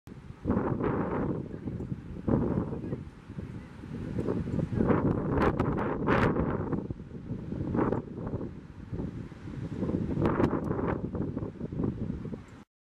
Tecnologia: Canon Sx 520 Full HD , teste de zoom em Torres
Torres, praia do litoral norte do Rs \ Brasil.
Zoom óptico: 42x
Digital: 4x ?
Outro: 168x
16 MP